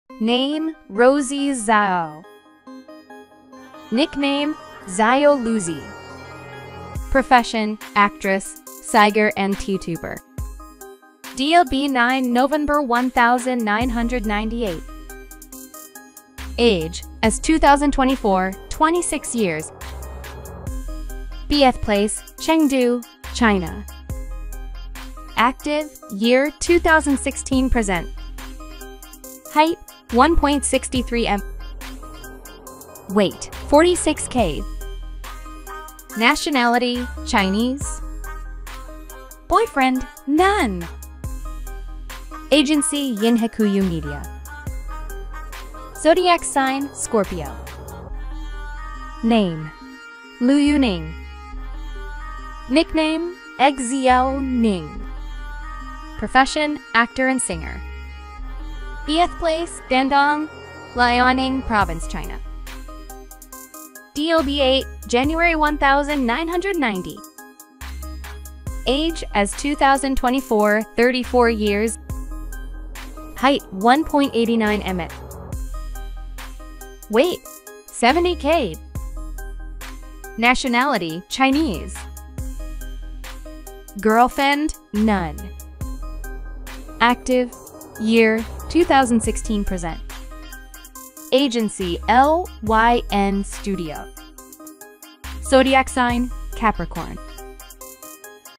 Zhao Lusi And Liu Yuning (The Story of Pearl Girl) Real Life Partner 2024
Name: Rosy Zhao

Nickname: Zhao Lusi

Profession: Actress, Siger & Tiutuber

DOB: 9 Novenber 1998

Age: As 2024 (26 Years)

Biethplace: Chengdu, China

Active: Year: 2016 Present

Weight: 46kg

Nationlity: Chinese

Boyfriend: None

Agency: Yinhekuyu Media

Zodiac Sign: Scorpio

Name: Liu Yuning

Nickname: Xiao Ning

Profession: Actor & Singer

Biethplace: Dandong, Liaoning Province China

DOB: 8 January 1990

Age: As 2024 (34 Years)

Height: 1.89 m

Weight: 70kg

Nationlity: Chinese

Girlfend: None

Agency: LYN Studio